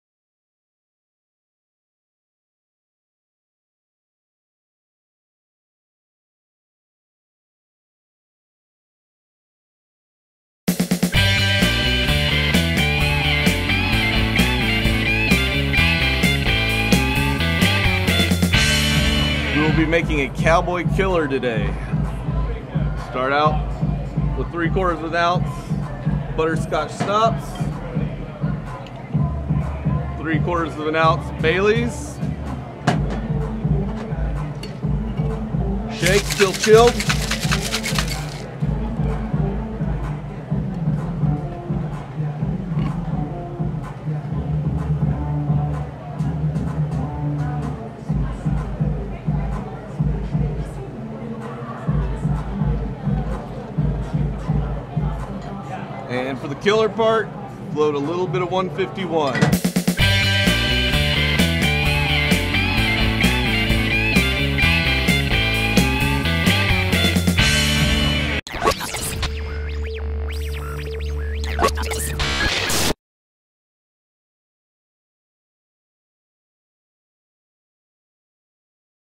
HOW TO KILL A COWBOY - "IT'S FRIDAY NIGHT" SERIES - BRANDIN' IRON
Documentary Film Maker Zeno Pierre visits a local night club called the Brandn' Iron and is treated to the making of a mixed drink called the Cowboy Killer. The Brandin' Iron plays the latest dance music but specializes in country western line dancing. The woman are hot. Awesome place!